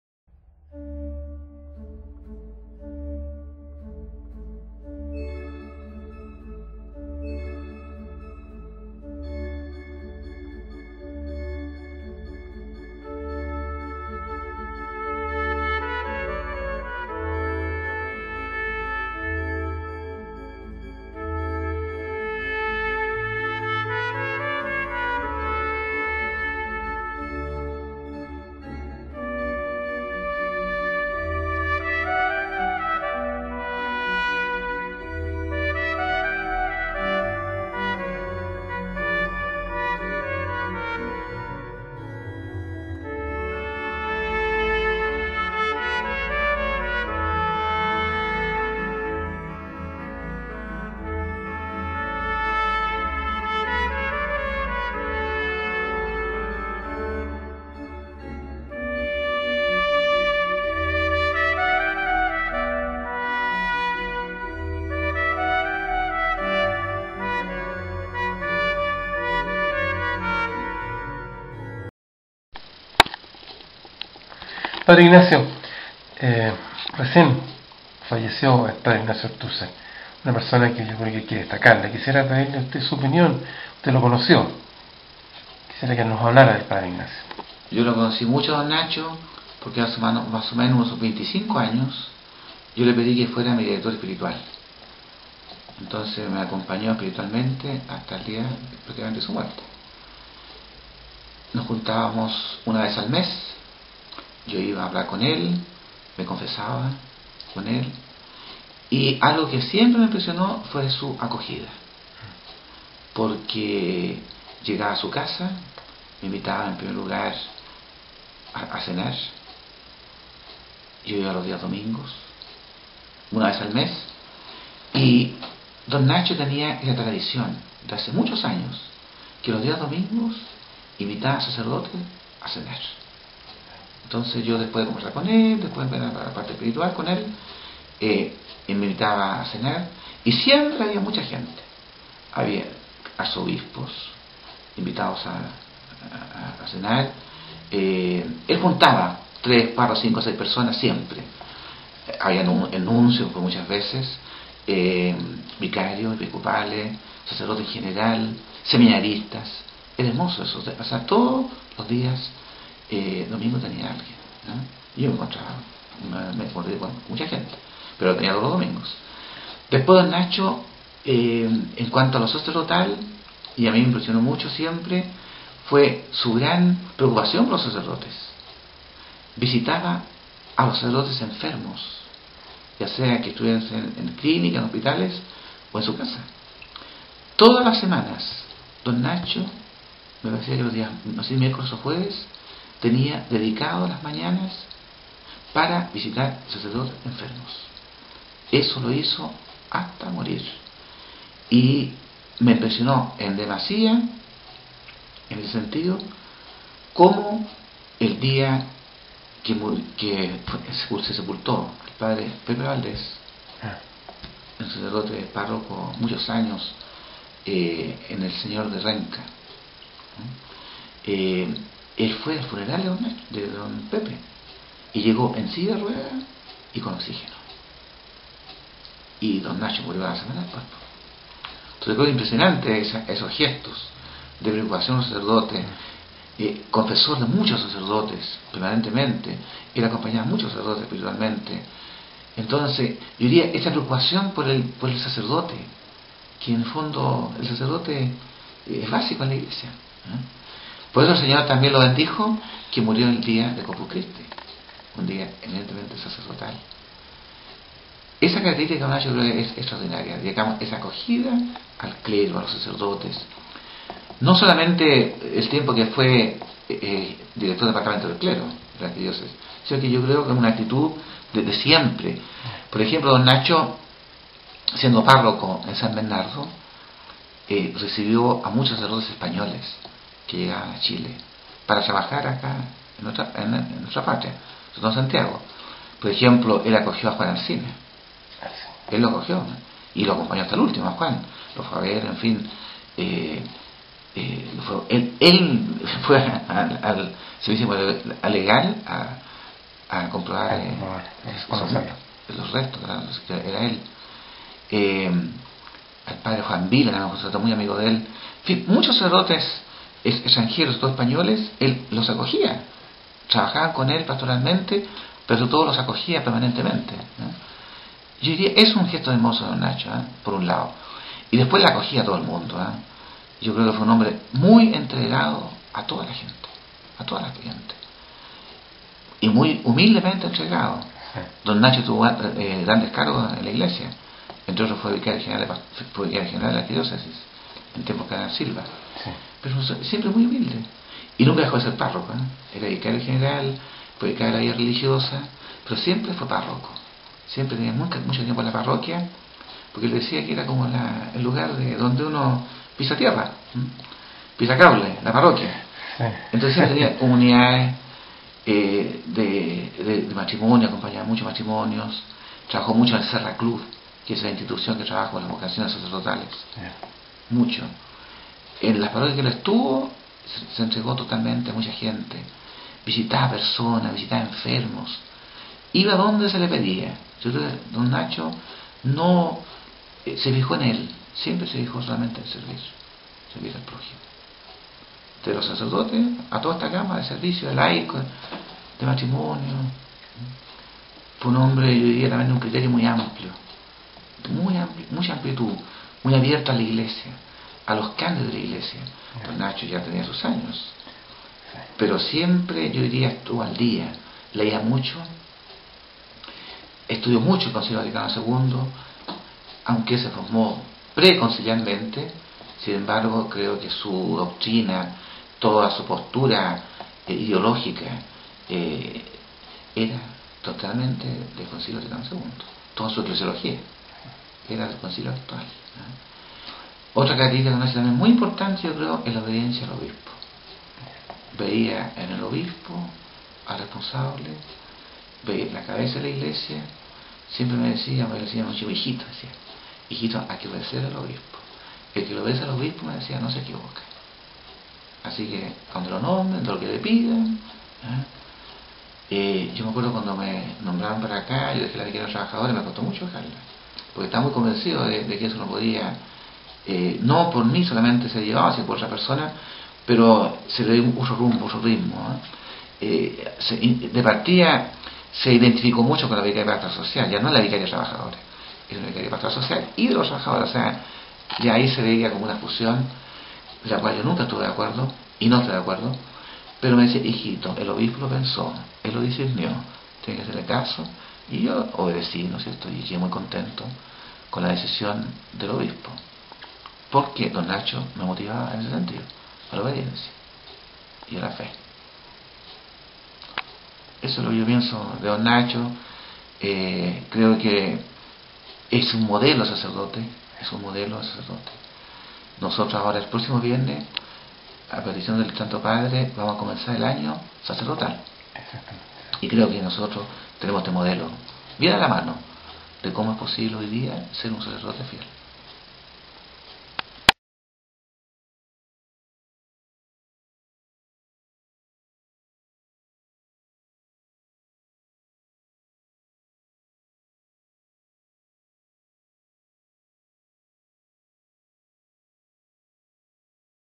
Recordando al P Ignacio Ortuzar Rojas
Un recuerdo y homenaje a este destacado sacerdote de la Arquidiócesis de Santiago.
Lo recuerda el P. Ignacio Muñoz, actual Párroco de San Francisco Solano, de la Zona Oriente de Santiago